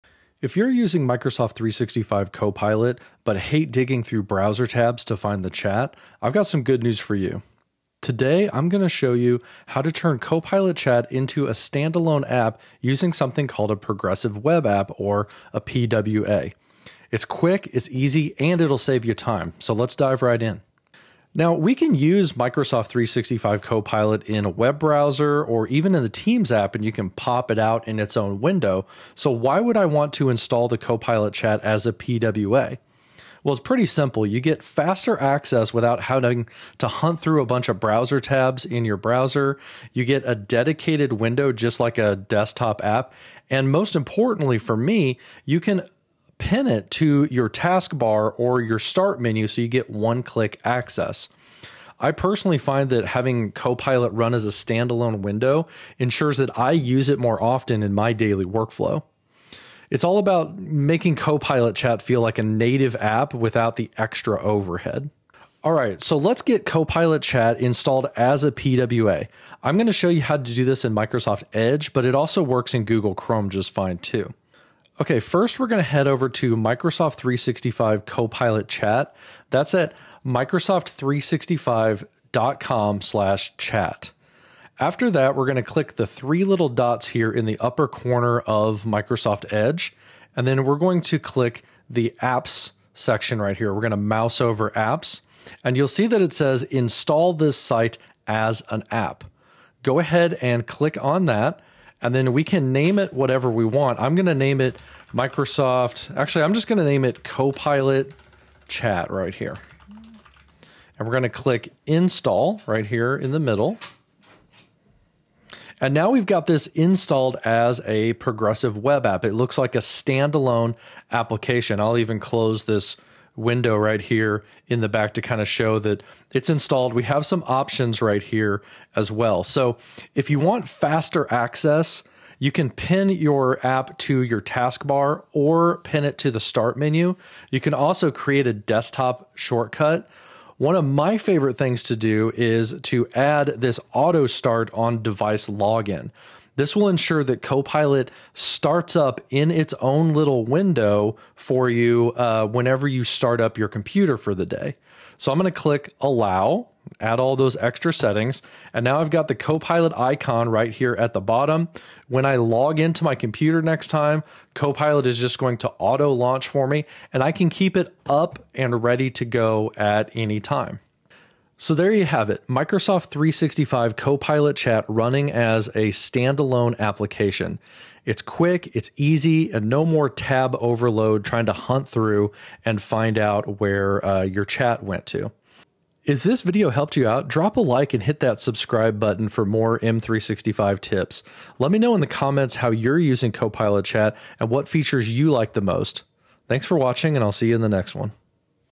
How to Install Microsoft 365 Copilot as an 'App' for Easy Access
🚀 Get to Microsoft 365 Copilot Chat Faster – Install it as a PWA!
Want Copilot Chat just a click away? In this quick tutorial, I’ll show you how to install Microsoft 365 Copilot Chat as a Progressive Web App (PWA). With this simple setup, you’ll have Copilot running as a standalone app—ready when you are!

✅ What You’ll Learn:
• What a PWA is and why it matters
• Step-by-step installation in Edge and Chrome
• How to pin Copilot Chat for even quicker access
• Pro tips for customizing your PWA experience